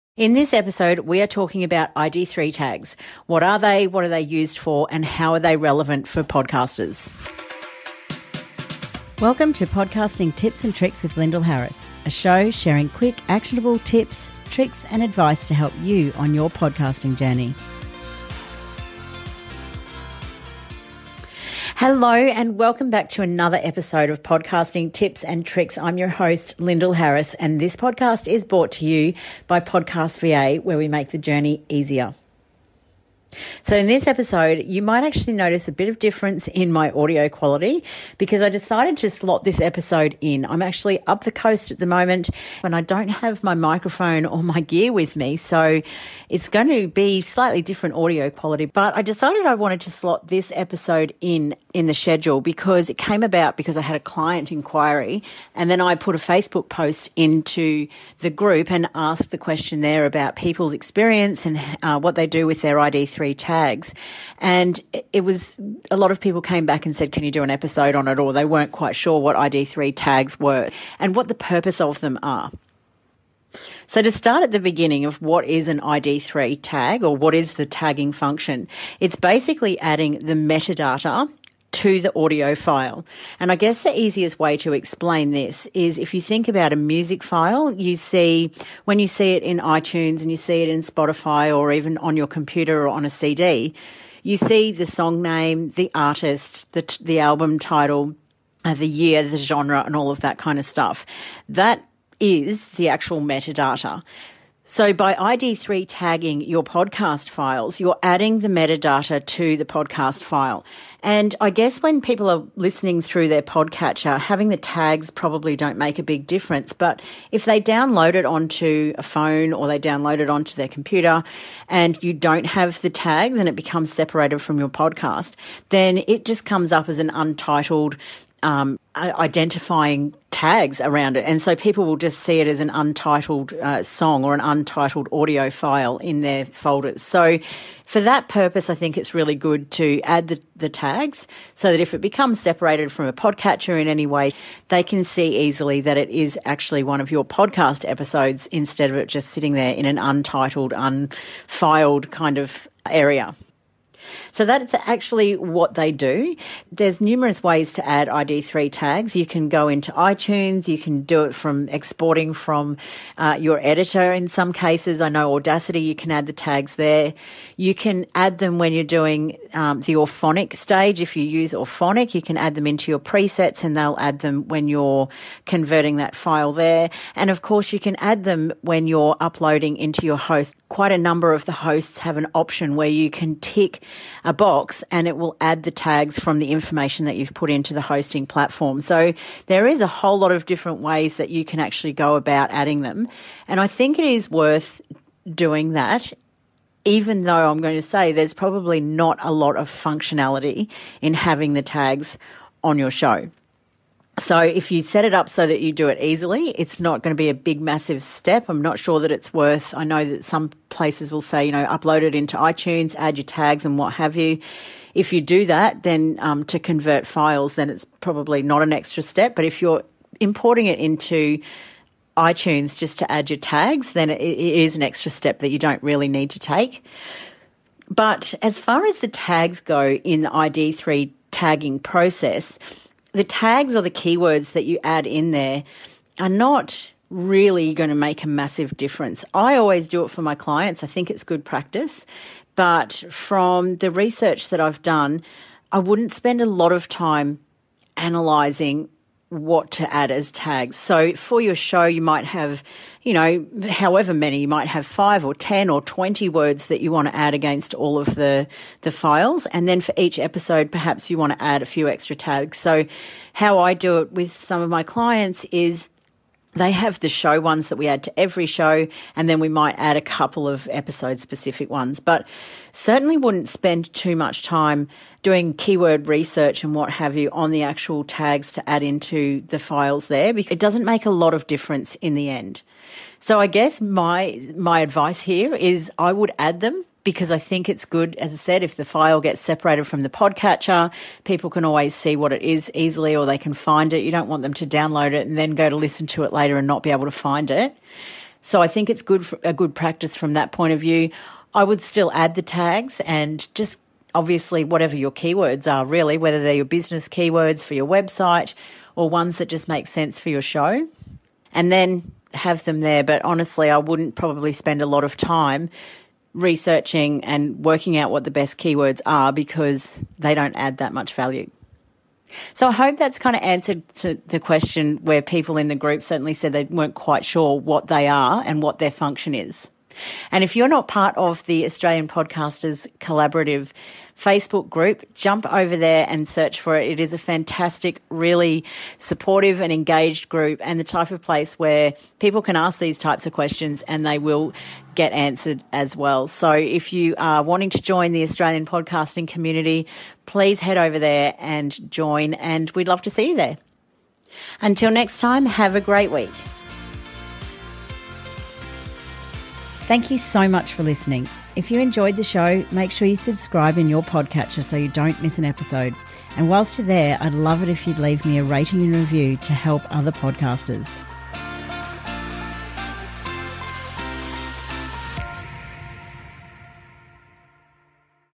ID3 Tagging
In this show, I'm talking about ID3 tags. This topic came up from a client's question and then prompted some discussion in the Australian Podcasters Collaborative Facebook Group.

In this episode:

  •  What is an ID3 tag
  •  What is their purpose.